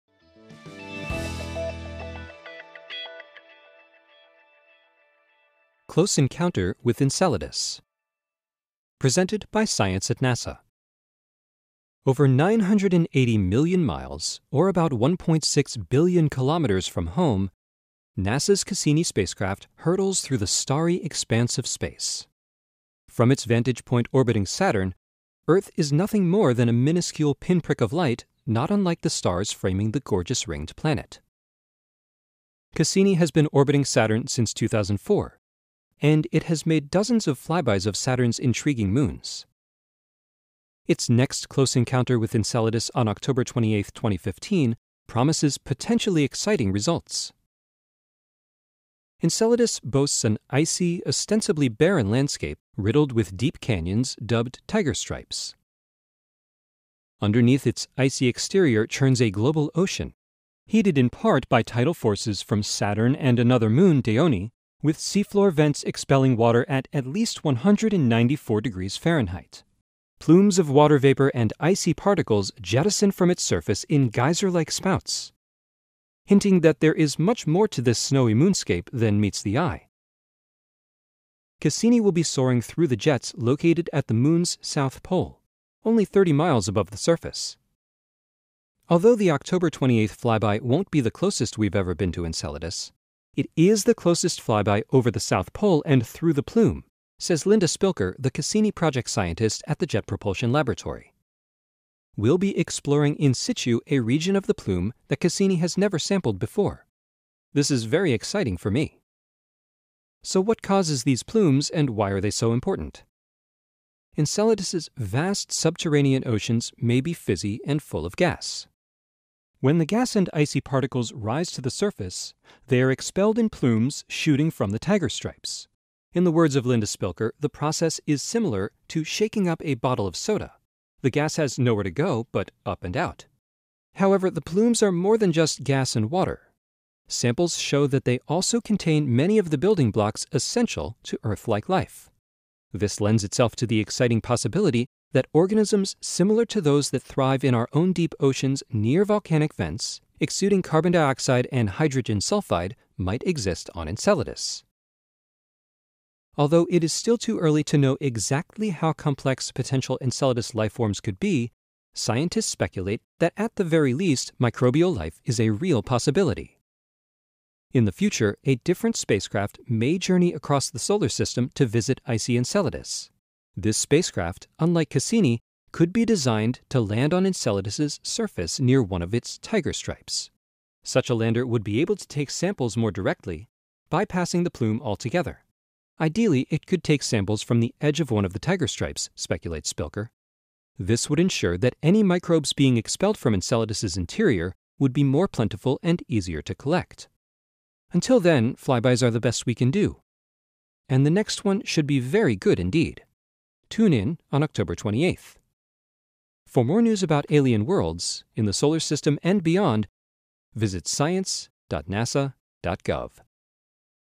ScienceCasts: Close Encounter with Enceladus
NASA's Cassini Spacecraft is about to make a daring plunge through one of the plumes emerging from Saturn's moon Enceladus.